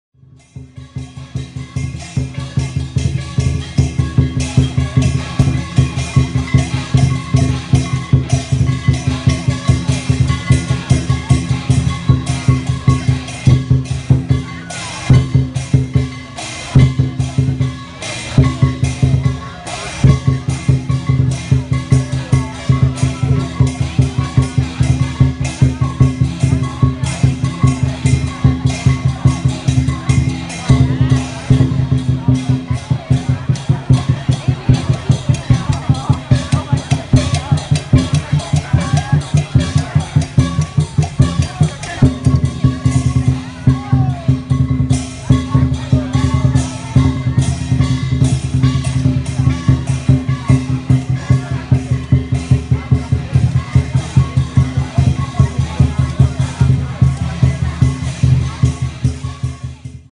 Betel Box chases the Fire Dragon
In conjunction with Mun San Fook Tuck Chee's 150th Anniversary, a Fire Dragon Dance (Singapore's one & only) commemorates the occasion.

"The practice of parading the burning dragon has its origins in Guangdong – the origins of many in the community. Made of straw that has been imported from China, such a dragon would previously have been constructed for the feast day of the temple’s principal deity and sent in flames to the heavens.  In more recent times, such straw dragons would be paraded on an average of once every three years.  This particular dragon, which made for a more recent Chingay Parade, is not burnt but set alight only by the placement of joss sticks on its body." - The Long and Winding Road.

Witness the ceremony as Betel Box Explore.

11th September 2016.